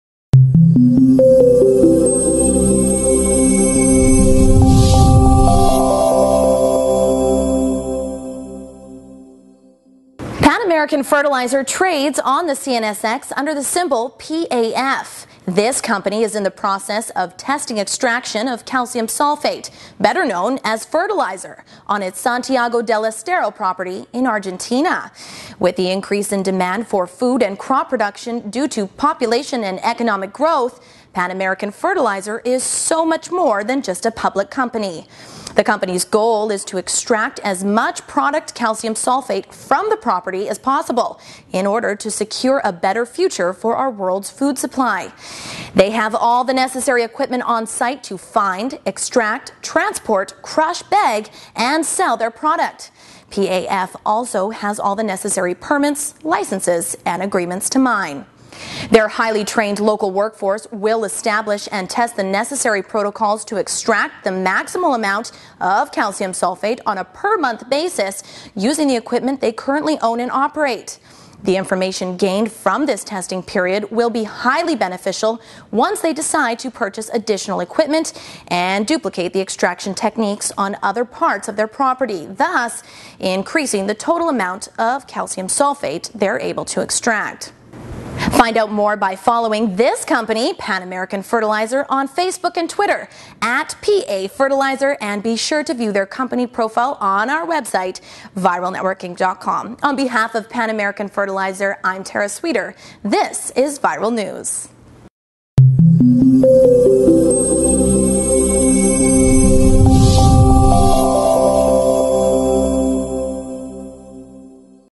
Pan American Fertilizer (CNSX: PAF) Intro Video - Viral News
Pan American is a Canadian Company dedicated to providing fertilizer to a growing global market. The company is focused on the extraction of a specific type of fertilizer called Calcium Sulphate (also referred to as "Agricultural Gypsum"). To ensure long term development and increase shareholder value, Pan American currently plans to significantly expand its current operational objectives while expanding its asset base by acquiring additional Calcium Sulphate and other Fertilizer related assets.

When used as a fertilizer and as a soil remediator, Calcium Sulphate is a soft sulfate mineral composed of Calcium Sulfate Dihydrate which is extremely rich in Sulphur and Calcium. When dissolved in water, the mineral becomes Calcium and Sulphate Sulphur ions, both of which are required nutrients for plants. Calcium Sulphate plays a vital role in establishing and maintaining good chemical balance in soil, water and plants, specifically with healthy root development. Ultimately, Calcium Sulphate increases overall crop quality and yields.

This is a paid awareness campaign, please read our full disclaimer